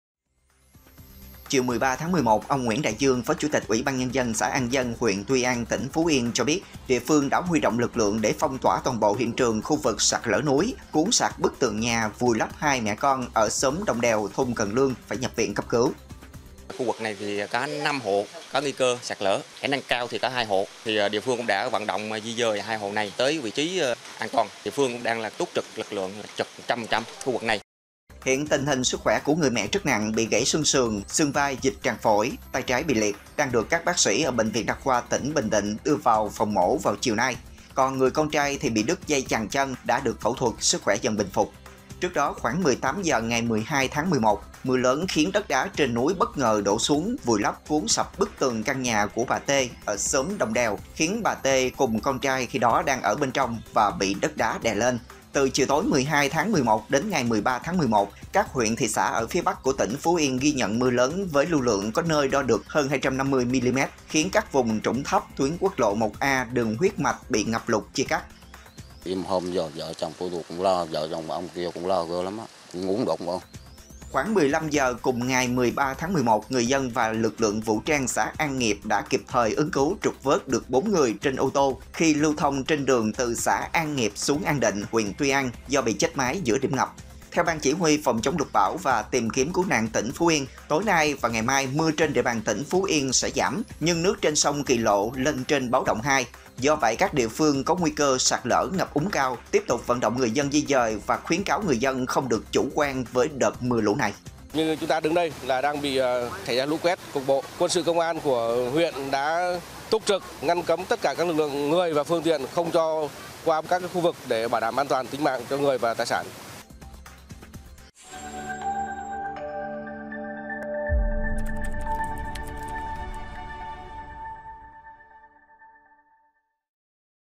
Phú Yên mưa lớn gây sạt lở, ngập lụt, chia cắt nhiều nơi
Từ chiều tối 12-11 đến ngày 13-11, các huyện, thị xã ở phía Bắc của tỉnh Phú Yên ghi nhận mưa lớn với lưu lượng có nơi đo được hơn 250mm, khiến các vùng trũng thấp, tuyến quốc lộ 1A, đường huyết mạch bị ngập lụt, chia cắt.